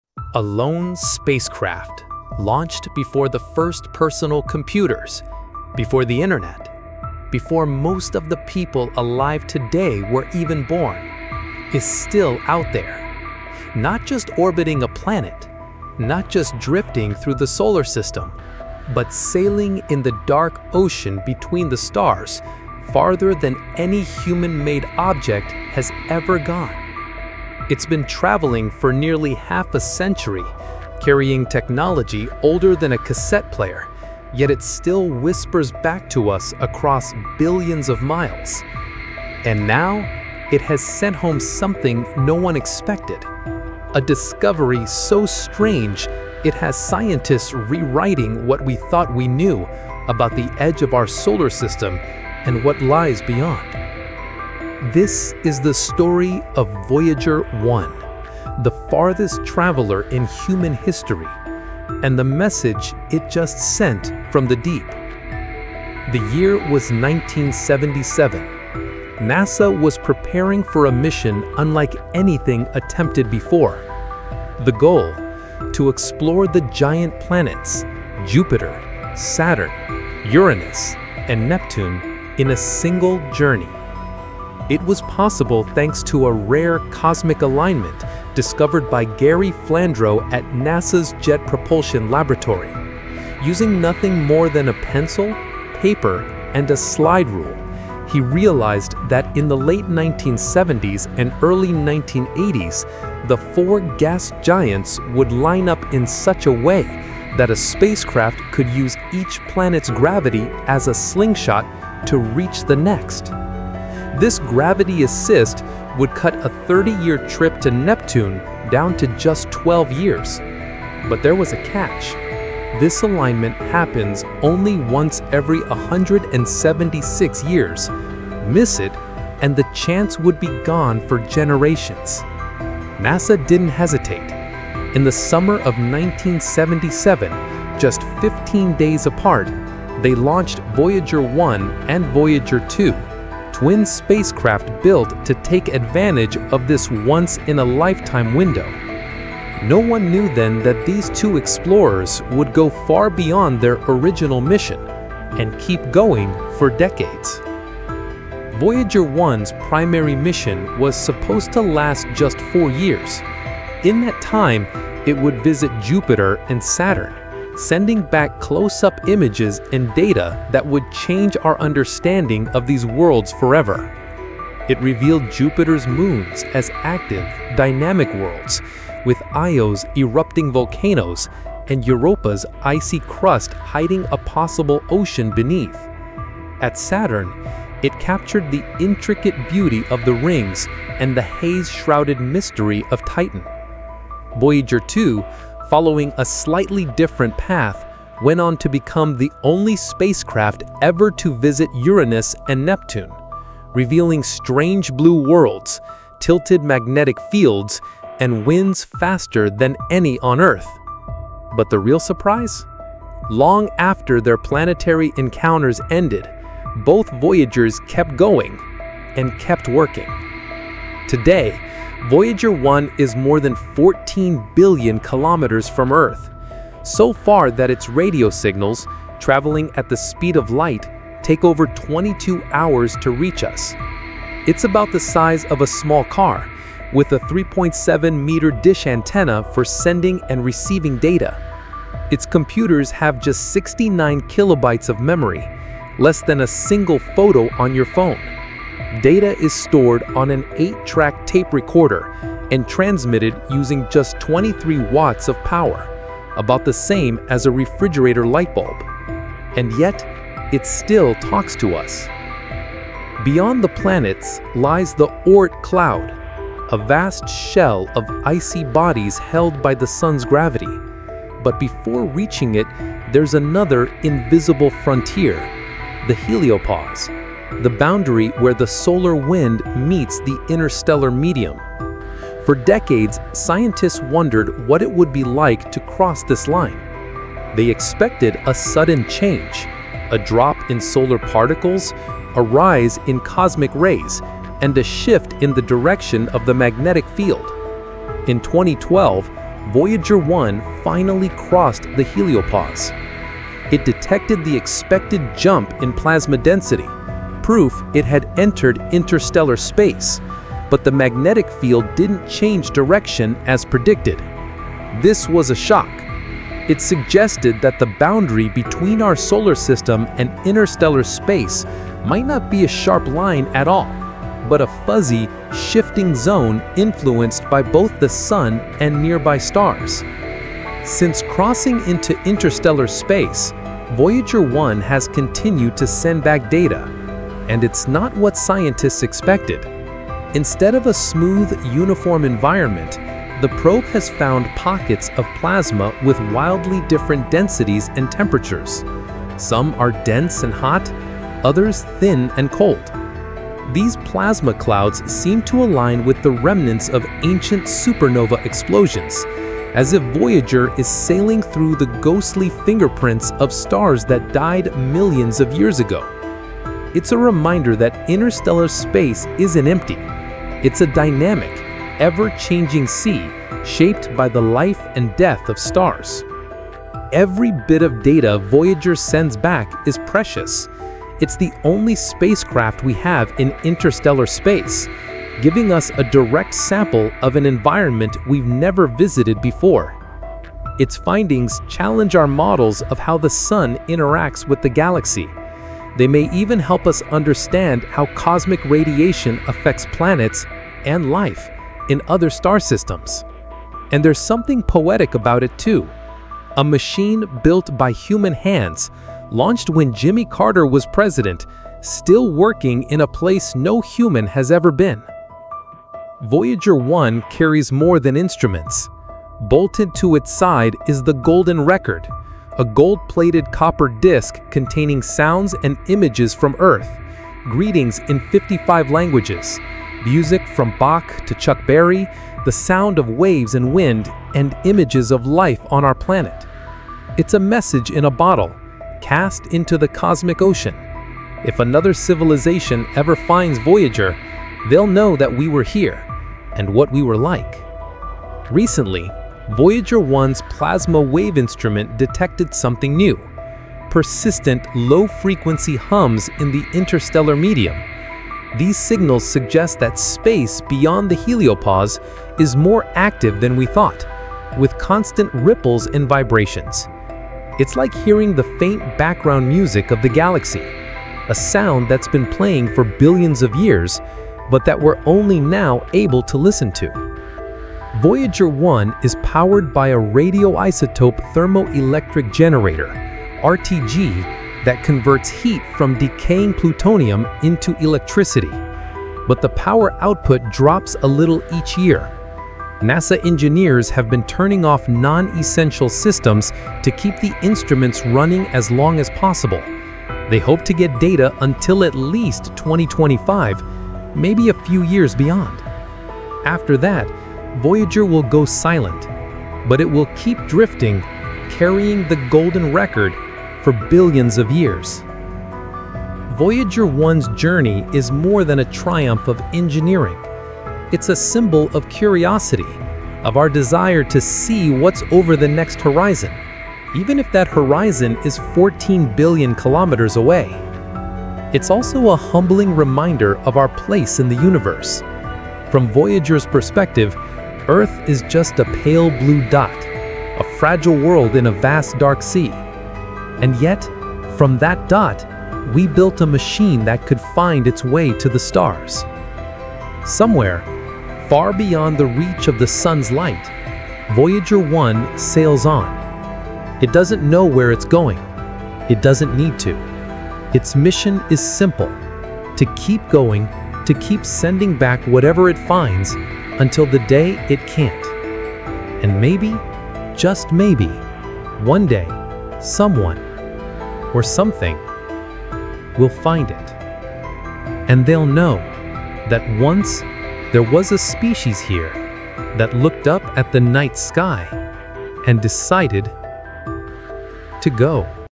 Voyager1's 46 Year Journey Ends with IMPOSSIBLE Discovery in Space
🚀 After 46 years in space, NASA’s Voyager 1 — the most distant human‑made object — has just sent back data that scientists are calling “impossible.”

Launched in 1977, Voyager 1 was only meant to explore Jupiter and Saturn. Instead, it has traveled over 14 billion kilometers, crossed into interstellar space, and kept sending discoveries that defy our understanding of the cosmos.

In this video, you’ll discover:

How Voyager 1’s journey began with a once‑in‑176‑year planetary alignment

The incredible close‑up encounters with Jupiter, Saturn, and their mysterious moons

What happened when Voyager 1 crossed the heliopause into interstellar space

The shocking plasma and magnetic field data that scientists never expected

Why this “impossible” discovery could rewrite what we know about the galaxy

From ancient supernova fingerprints to the faint hum of the interstellar medium, Voyager 1’s latest findings are more than just science — they’re a message from the edge of the Sun’s influence.

🌌 Is this the final chapter of Voyager 1’s mission, or the beginning of a new mystery?